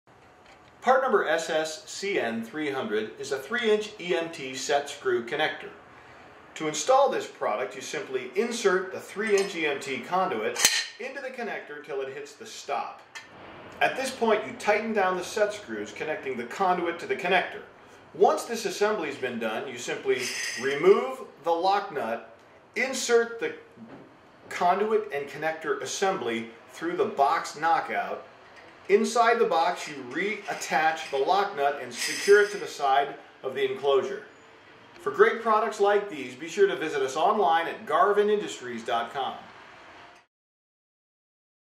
Set Screw Connector: 3" Uninsulated Zinc Plated Set Screw Connector SSCN300
- 3" uninsulated set screw electrical connectors are used in dry locations to bond 3" EMT thinwall conduits to electrical junction boxes or enclosures. Garvin Industries' uninsulated steel set screw connectors provide concrete tight connections when taped and properly secured with the included locknut, and are rated for electrical applications above 600 volts. Their heavy gauge steel construction maintains mechanical protection and solid grounding of the conduit to the designated electrical junction box or enclosure. Additionally, set screw connectors are zinc electroplated for excellent corrosion protection. Set screw connectors are also made with a versatile combination head screw that can securely fasten a conduit to an electrical junction box using a slotted, Phillips, or Robertson head screw driver.